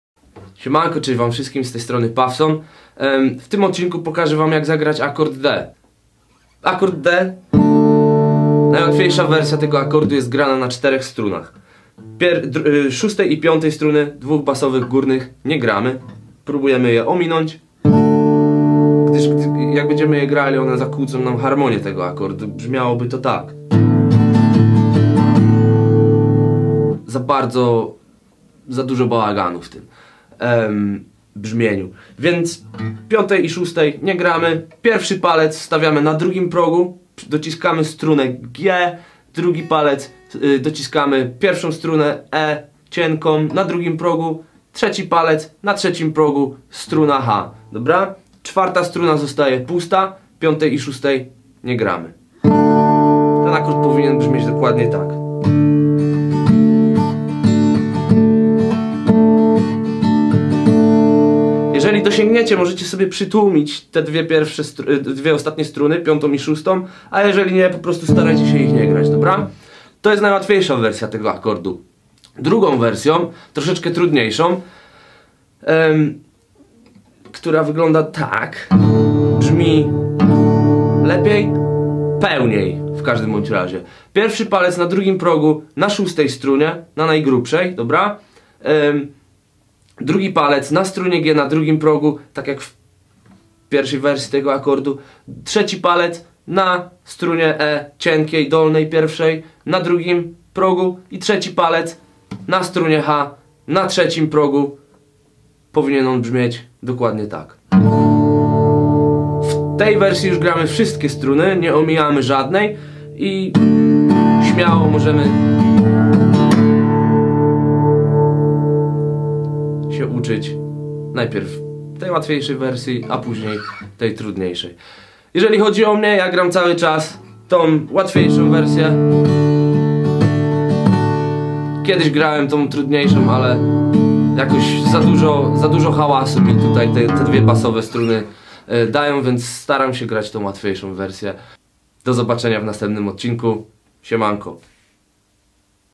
JAK ZAGRAĆ AKORD D-dur NA GITARZE
Dzisiejszy filmik nauczy nas jak zagrać akord D- dur na gitarze.

Przedstawiam dwie wersje tego chwytu, łatwą i troszkę trudniejszą.

Od Was zależy którą lepiej Wam zagrać, obie wersje brzmią bardzo podobnie.

Zostańcie ze mną, a gwarantuję Wam, że będzie bardzo ciekawie.

Życzę miłej nauki i jeszcze milszego grania!!!

Jak ten film był dla Was pożyteczny to poprosze o łapkę w górę i podzielenie się z innymi, którzy też chcą się nauczyć grać na gitarze.